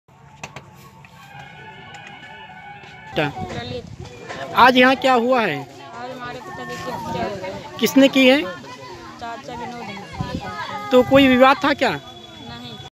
रायबरेली: दिव्यांग भाई को बेरहमी से पीटा, मौत
रायबरेली: शिवगढ़ थाना क्षेत्र में एक युवक ने अपने दिव्यांग भाई की बेरहमी से पिटाई कर दी, जिससे उसकी मौत हो गई. मौके पर पहुंची पुलिस ने घटनास्थल की छानबीन कर आरोपी को पकड़ने का आश्वासन दिया है.